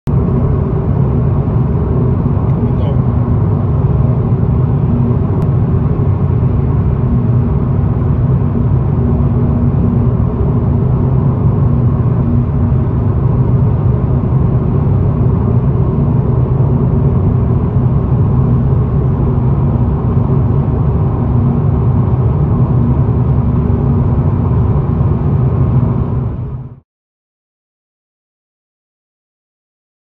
Moon 🌙 looks like a mini Cloud ☁️
While driving I got this glimpse of  Moon 🌙 looking like a mini Cloud ☁️  and just want to share 😎